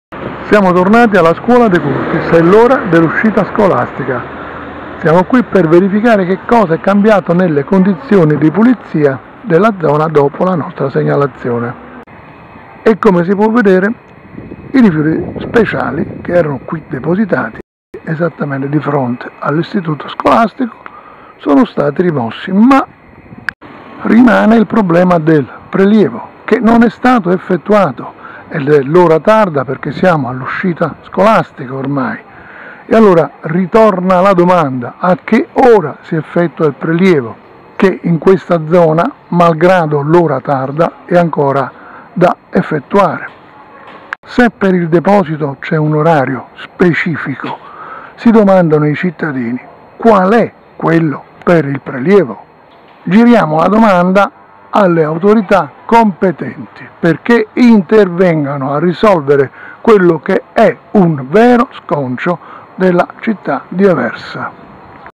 Aversa (CE) - Via Amendola, proteste su prelievo rifiuti (23.06.12)
- Aversa (Caserta). Ancora proteste da via Amendola per il mancato rispetto degli orari di prelievo dei rifiuti che restano nei cassonetti collocati all'ingresso dei condomini fino alla tarda mattinata e , spesso, anche oltre, creando un panorama da discarica in tutta la zona. Dopo aver chiesto, con successo, aiuto alle telecamere di Pupia, perché si eliminasse lo sconcio del deposito selvaggio fatto accanto al cassonetto per il vetro, diventato punto di attrazione per sacchetti e buste cariche di rifiuti di ogni tipo, trasformando di fatto in discarica a cielo aperto l'area al confine con l'istituto scolastico "Antonio De Curtis", i residenti avevano rinnovato l'appello perché fossero rimossi anche i rifiuti speciali, costituiti da rotoli a base di bitume, ammucchiati a due passi dalla scuola che erano stati lasciati sul posto da chi era intervenuto a rimuovere la discarica accanto al cassonetto per il vetro. Nell'occasione avevano sollecitato gli organi preposti non solo a continuare nella rimozione quotidiana, così da abituare gli incivili a non depositare più in quel punto rifiuti diversi dal vetro, ma anche a disporre controlli perché i cassonetti condominiali fossero svuotati nella prima mattinata, in modo da ridare dignità a una zona residenziale confinante con un istituto scolastico frequentato da centinaia di bambini. Dopo le belle parole dette in conferenza stampa dal rappresentante della ditta incaricata del servizio di igiene urbana, pagata profumatamente dai cittadini, sono stati ascoltati? Qualcuno ha dato loro risposta?

di Antonio Arduino (23.06.12)